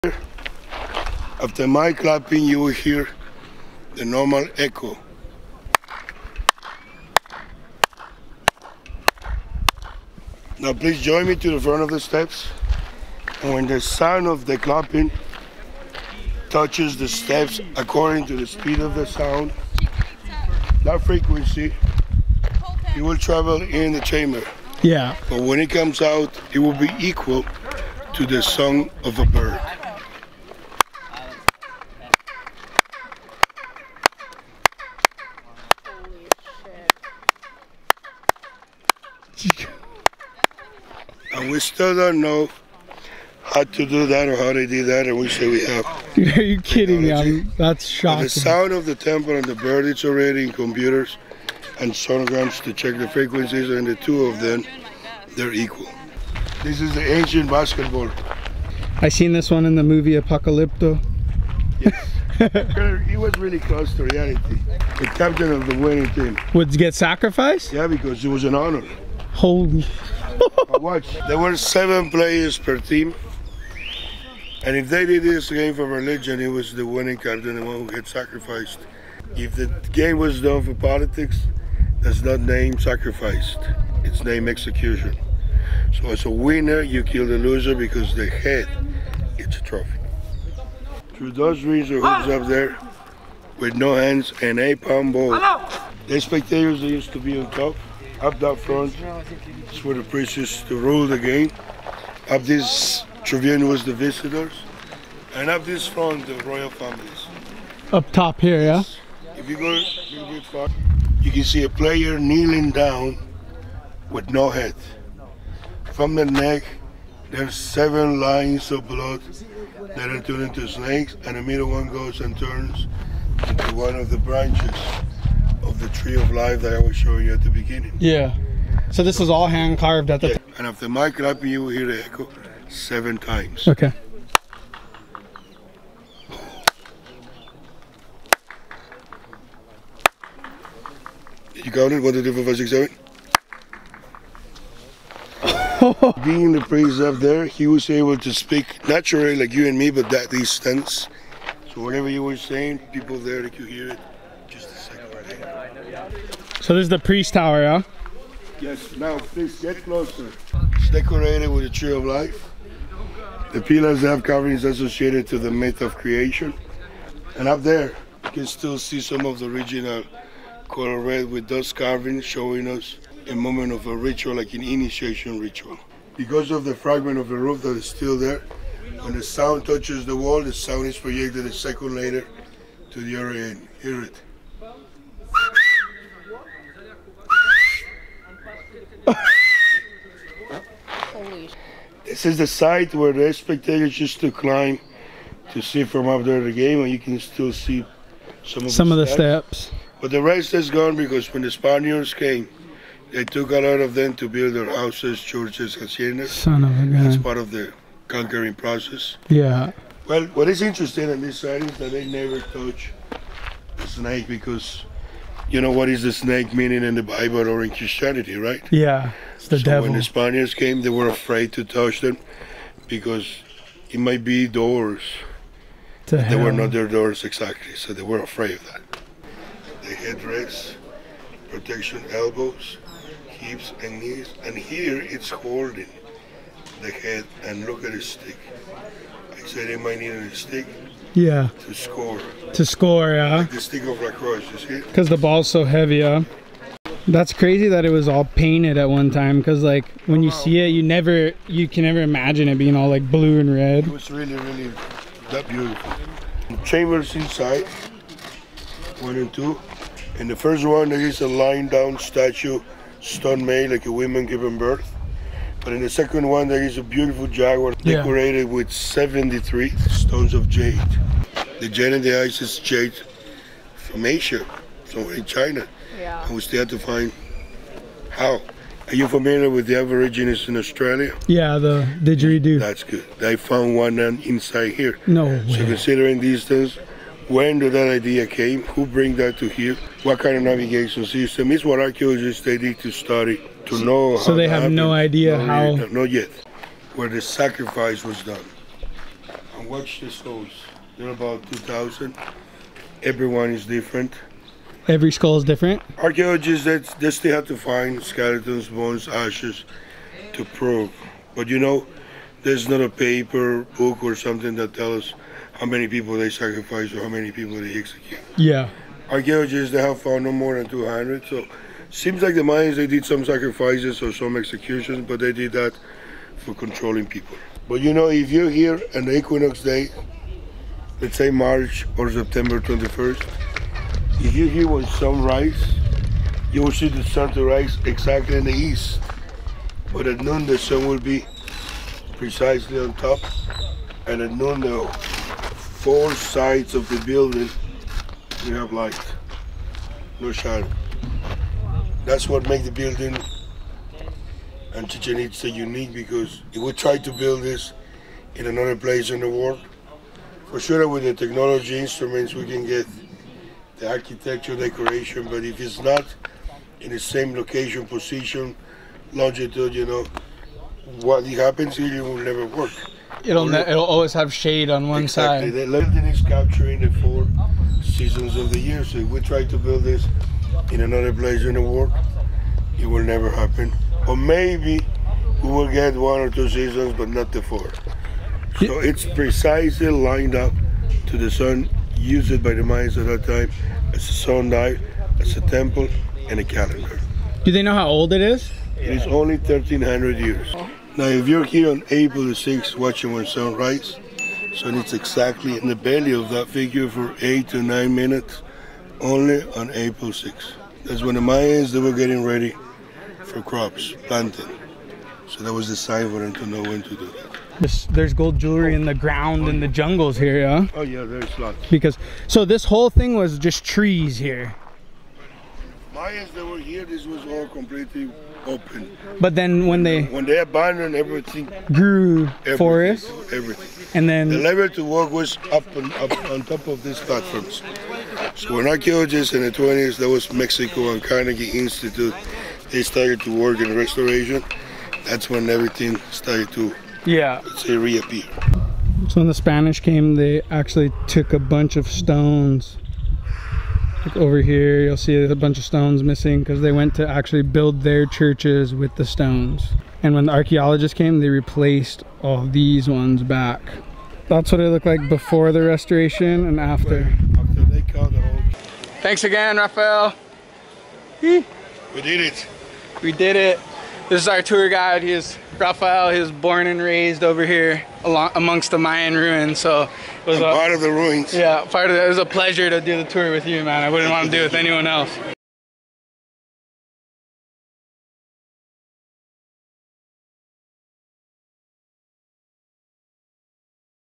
Mayan Mystery of Chichen Itza
Mayan Mystery of Chichen Itza. Highlights from our Chichen Itza walking tour. When we were entering Chichen Itza we had a guide offer to give us a paid private tour. At first I almost passed up the offer but I liked the guys personality so much that we decided to do it. Touring Chichen Itza with this private guide was the highlight of our trip and something I'll never forget. The acoustics inside the area are mind blowing! The Mayan culture is amazing... Watch this video to see why we left the place in awe!

Chichen Itzá Tour
Chichen Itzá walking Tour
Mexico
valladolid mexico
Mayan ruins
mayan people